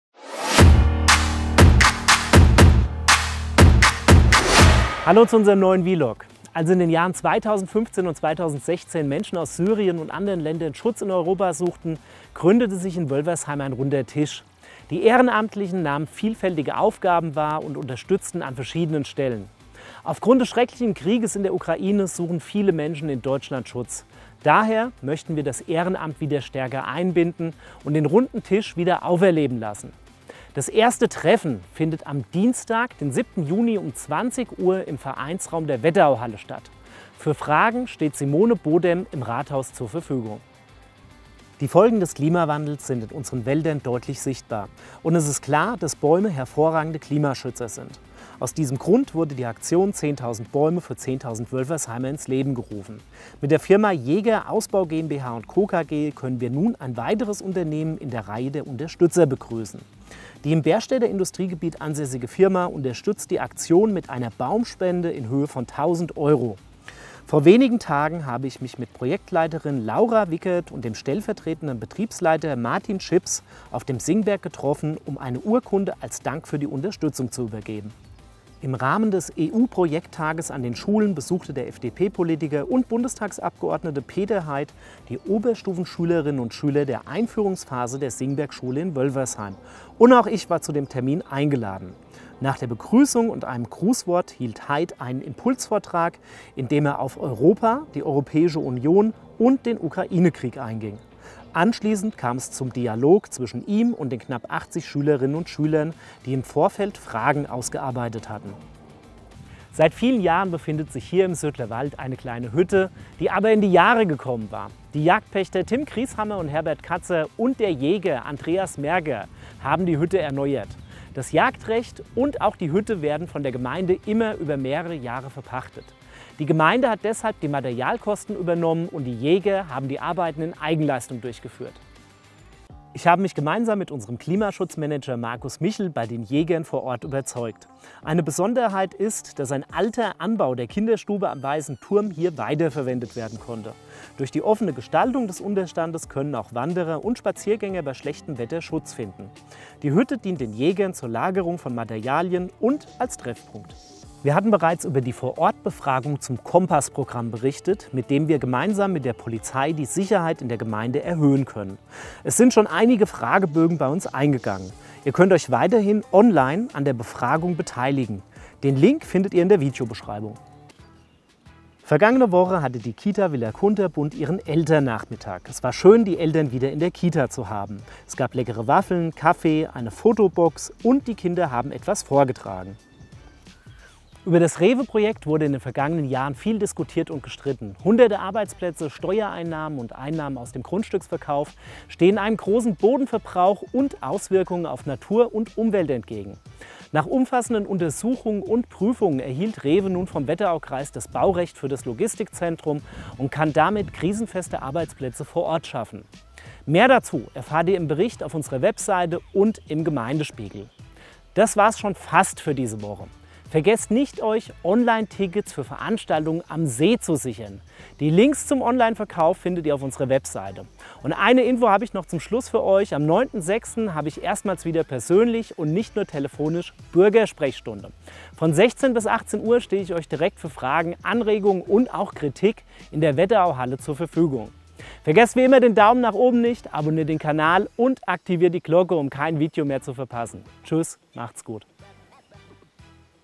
Wölfersheim - VLOG #130
In dieser Woche gab es eine Baumspende, eine Baugenehmigung, ein Kitafest und einiges mehr. Wie üblich berichten wir kurz und knapp über alle aktuellen Themen in unserem VLOG.

00:00 Intro und Runder Tisch
00:40 Baumspende
01:18 Besuch Bundestagsabgeordneter Peter Heid
01:49 Jagdhütte
02:34 KOMPASS Befragung
02:50 Fest der Kita Villa Kunterbunt
03:03 REWE Baugenehmigung, Bürgersprechstunde und Vorverkauf